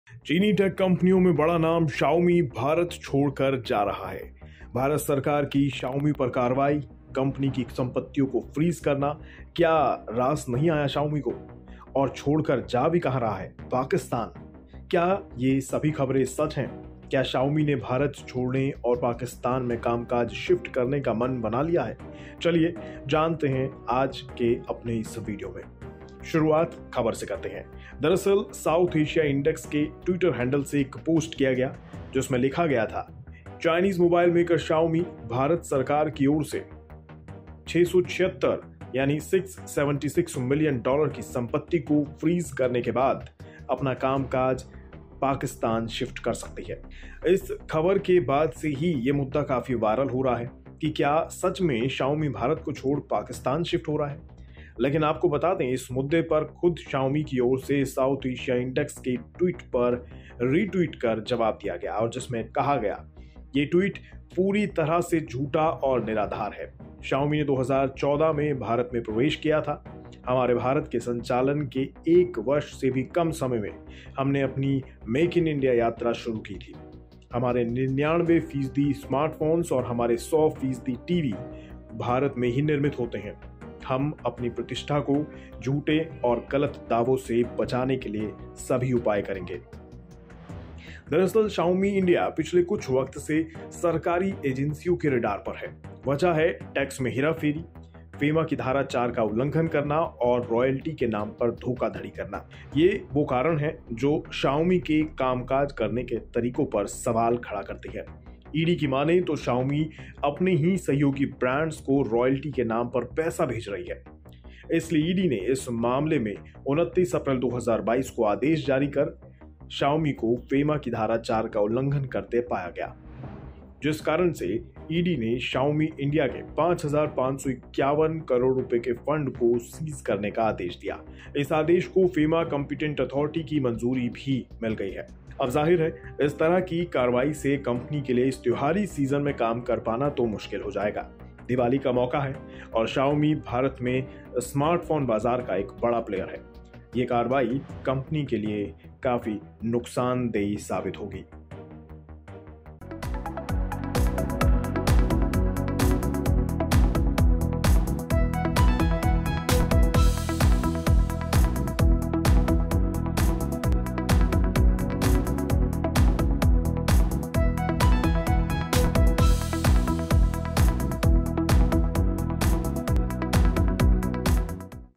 क्या Xiaomi सच में भारत छोड़कर Pakistan जा रहा है? |BIZ Tak
Xiaomi moving its India operations to Pakistan? Chinese mobile maker responds...